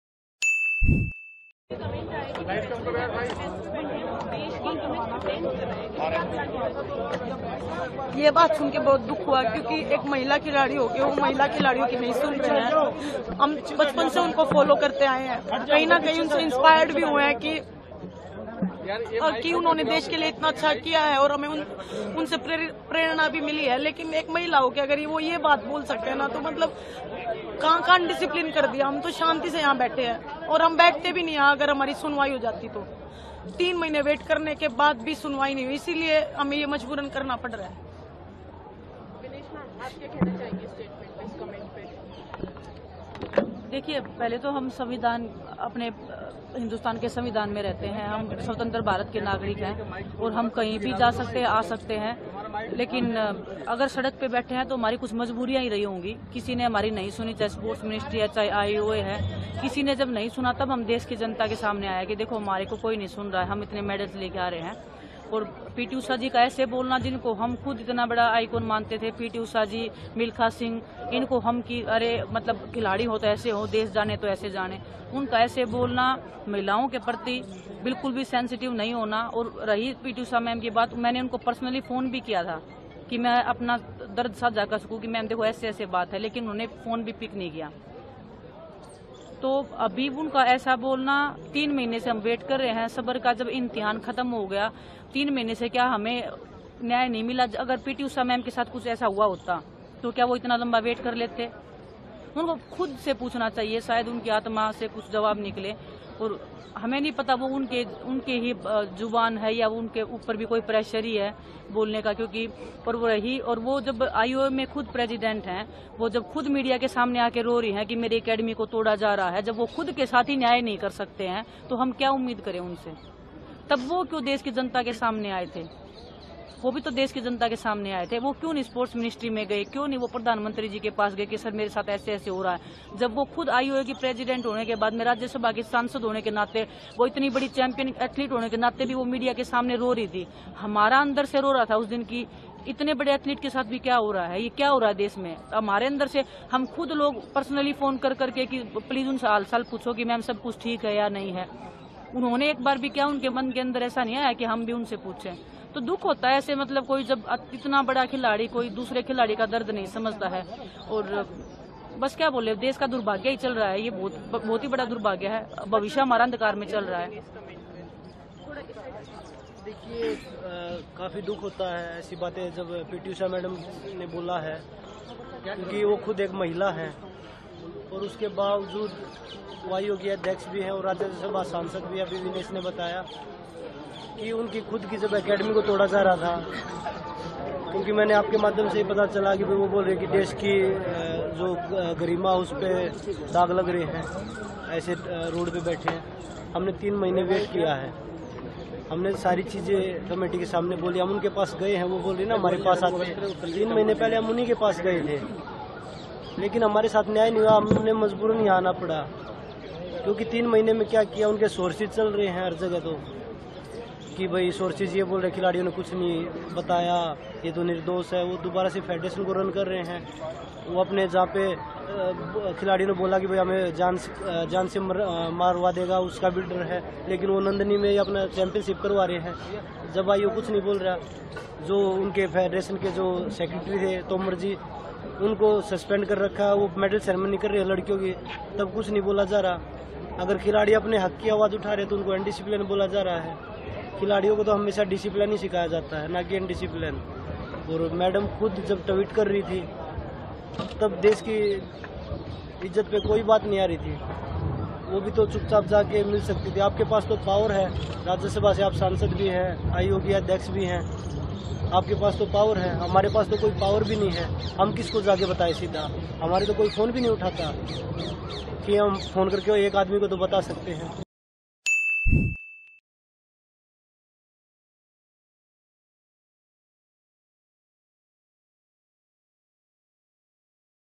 Wrestlers Protest | PT USHA के Comment पर फूट-फूटकर रोने लगीं Sakshi Malik। Jantar Mantar
Viral | Wrestler Protest पर PT Usha की टिप्पणी पर पहलवानों ने तीखी प्रतिक्रिया दी है. Wrestler Sakshi Malik PT Usha की टिप्पणी पर प्रतिक्रिया देते हुए फूट-फूटकर रोने लगीं. उन्होंने कहा कि मैंने मैम को फोन तक किया था, लेकिन उन्होंने मेरा फोन तक नहीं उठाया. साक्षी मलिक ने आगे कहा कि PT Usha मैम भी एक महिला हैं, क्या उनके साथ ऐसा हो रहा होता तो वो इतने समय तक चुप रहतीं? वहीं, Wrestler Vinesh Phogat ने कहा कि जब PT Usha मैम मीडिया के सामने आकर फूट-फूटकर रोने लगीं थीं, तब वो PM Modi से मुलाकात कर सकती थीं. उन्होंने मीडिया से क्यों कहा था कि उनकी एकेडमी को तोड़ा जा रहा है. वहीं, Wrestler Bajrang Punia ने कहा कि PT Usha मैम भी एक महिला हैं, उन्होंने हमारा दर्द समझने की कोशिश तक नहीं की. आपको बता दें कि Wrestler Protest में WFI President Brajbhushan Sharan Singh को हटाने की मांग की जा रही है. साथ ही उनके खिलाफ FIR दर्ज करने की भी मांग पहलवान कर रहे हैं.

Wrestlers have reacted sharply to PT Usha's comment on Wrestler Protest. Wrestler Sakshi Malik burst into tears while reacting to PT Usha's comment. He said that I had even called Mam, but she did not even pick up my phone. Sakshi Malik further said that PT Usha Mam is also a woman, if this was happening to her then she would have remained silent for so long? On the other hand, Wrestler Vinesh Phogat said that when PT Usha Mam started crying bitterly in front of the media, then she could have met PM Modi. Why did he tell the media that his academy was being demolished? Whereas, Wrestler Bajrang Punia said that PT Usha Mam is also a woman, she did not even try to understand our pain. Let us tell you that there is a demand for the removal of WFI President Brajbhushan Sharan Singh in Wrestler Protest. Along with this, wrestlers are also demanding registration of FIR against them.